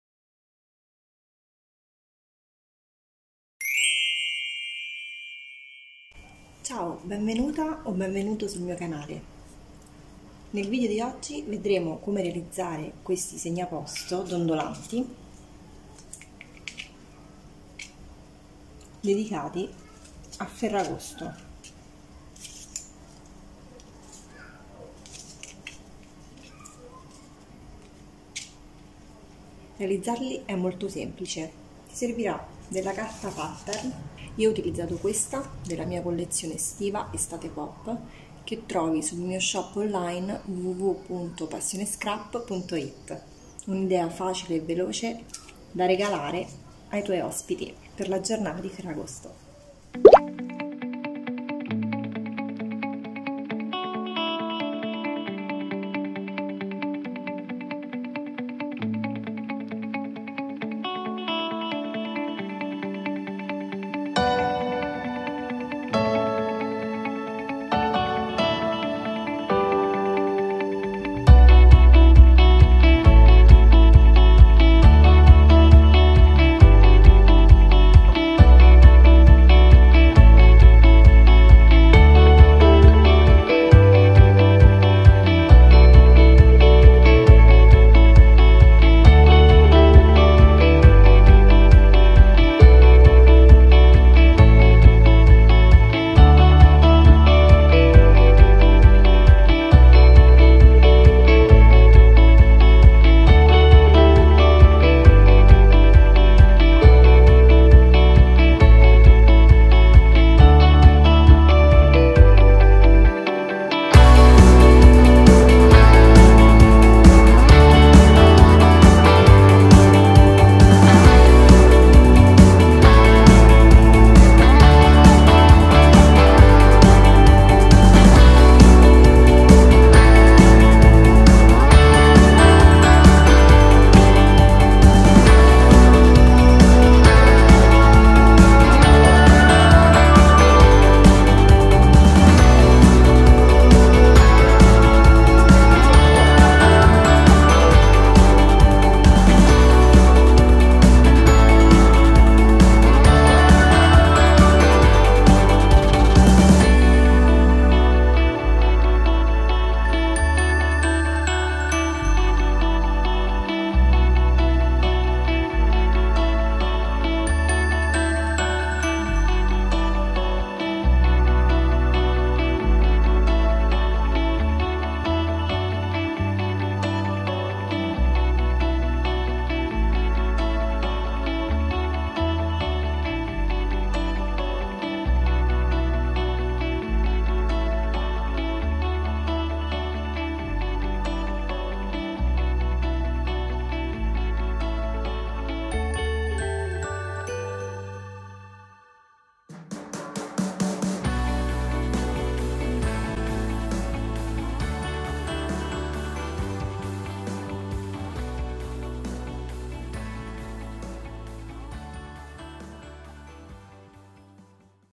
Crea Segnaposto "Oscillanti" per un Tavolo Estivo Festoso! 🍉🌼 | Tutorial Fai-da-Te
Ciao! In questo entusiasmante tutorial fai-da-te, ti mostrerò come creare dei carinissimi segnaposto "oscillanti" perfetti per dare un tocco festoso al tuo tavolo estivo! 🍉🌼

Se come me, adori l'estate e desideri rendere le tue giornate con amici e familiari ancora più speciali, questo progetto creativo è proprio ciò di cui hai bisogno. Utilizzerò la vibrante collezione "EstatePoP" di per dare vita a segnaposto unici e colorati.

🛍️ Per il materiale necessario, vi basteranno poche cose facilmente reperibili e un po' di creatività.  Non c'è bisogno di essere degli esperti per realizzare questi fantastici segnaposto!

Durante il tutorial, ti guiderò passo dopo passo, puoi coinvolgere tutta la famiglia e gli amici per rendere la preparazione dei segnaposto un'attività creativa da condividere insieme.

🎬 Non perdere l'opportunità di dare uno sguardo al mio video tutorial! Clicca sul pulsante di riproduzione e scopri come trasformare una tavola semplice in una festa estiva indimenticabile.

Se sei interessato ai materiali utilizzati nel tutorial, troverete il link per acquistare la collezione "EstatePoP" di PassioneScrap nella descrizione del video.

Sei pronto a dare vita a segnaposto unici e originali per rendere ogni occasione speciale? Allora, preparati a essere ispirato e a tuffarti in questo progetto fai-da-te! 🎨🌟